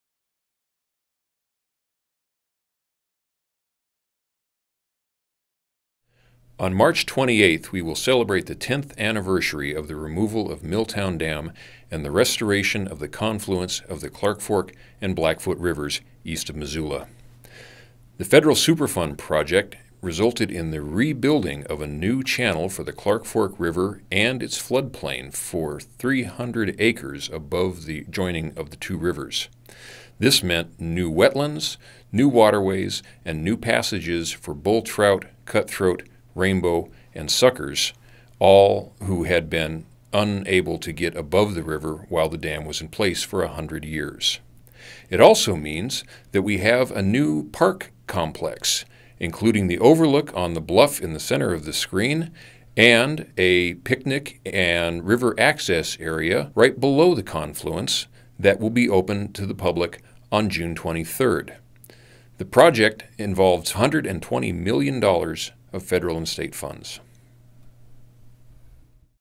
Rivers Run Free | 10 years after the removal of the Milltown Dam
Ten years ago on March 28, 2008, construction workers breached the Milltown Dam and allowed the Clark Fork River to flow freely for the first time in 100 years. Some $120 million from the state of Montana and federal government funded the restoration of the Clark Fork River's confluence and flood plain. Now native fish, plants and animals thrive in what was once the Milltown Reservoir.